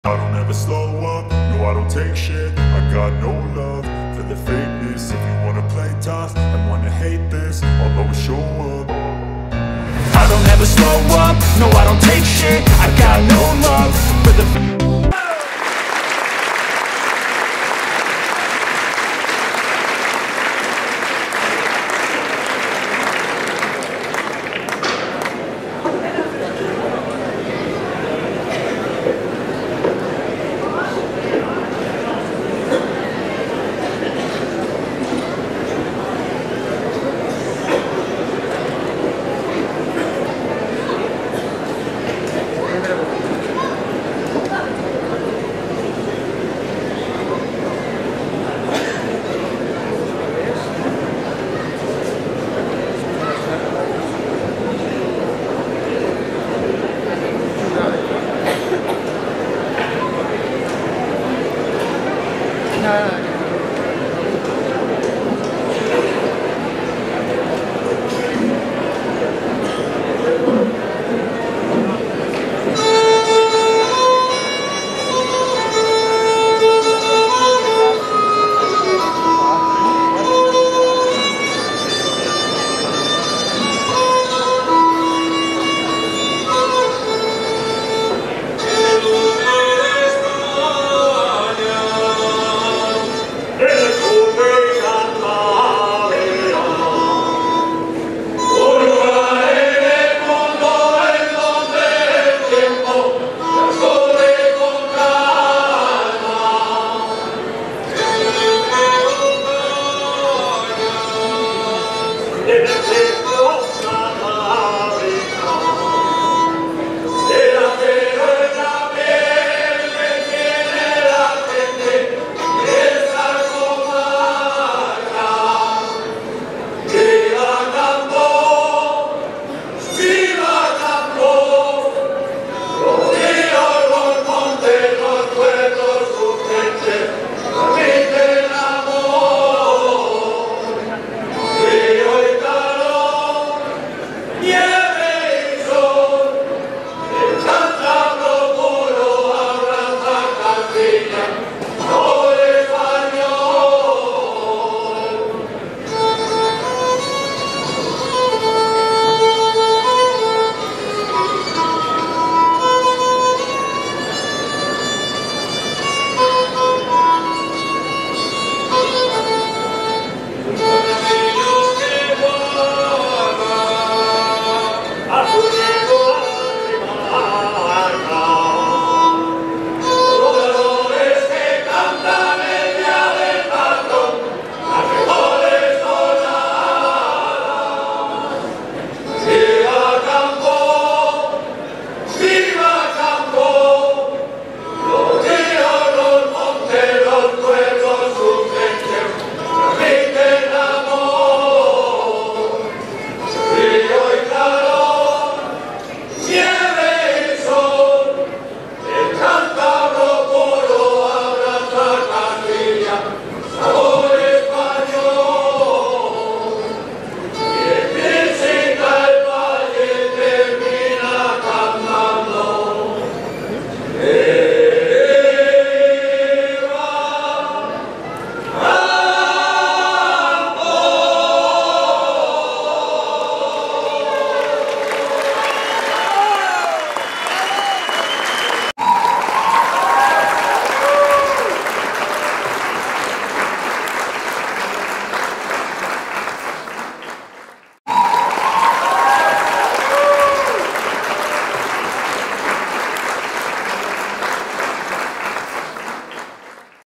Ronda La Esperanza de Requejo Tanos Torrelavega 4 Mayo 2024
El sabado 4 de Mayo del 2024, se hizo un homenaje postumo a: Julian Revuelta: El Malvis de Tanos. Estuvimos filmando a varios grupos.. Nando Agueros, Loa Hermanos Cosio, Miguel Cadavieco.. etc.